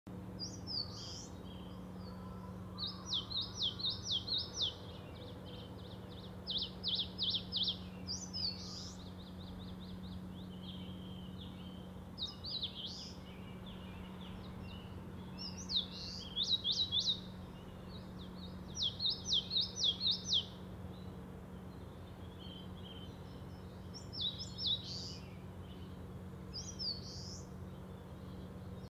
【動画素材 9055】センダイムシクイ | 北海道 斜里 | 4K UHD
動画素材『センダイムシクイ | 北海道 斜里 | 4K UHD』のサンプル映像です。